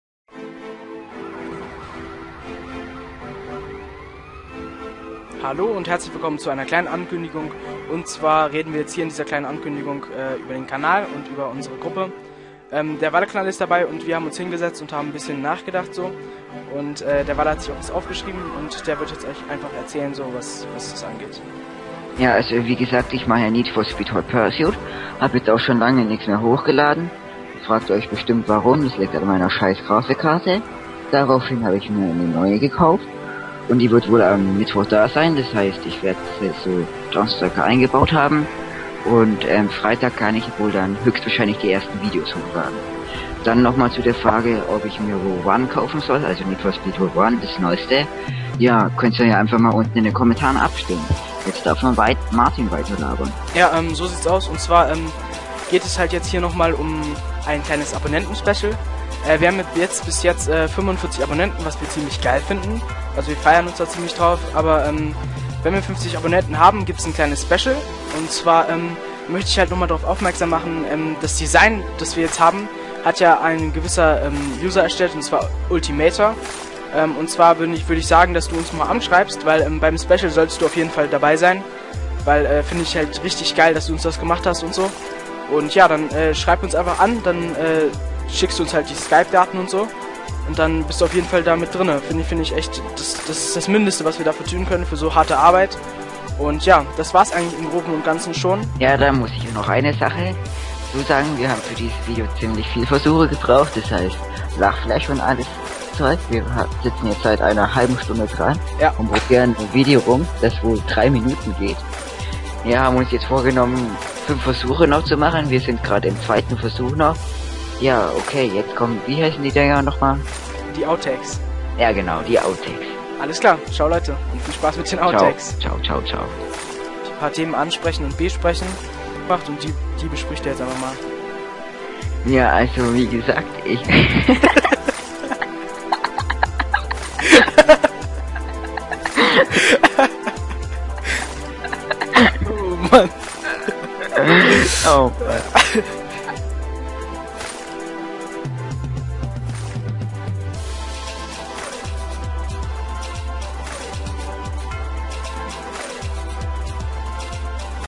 #1 Folge Mai Monatliche Ankündigungen [Deutsch][HD+]
So hier n paar Ankündigungen von und mit Valle und Marti:D
Viel Spaß

Hammer Soundtrack von DasistHanf :)
Hier n Link zu seinem Kanal.
Müsst ihr euch unbedingt gönnen.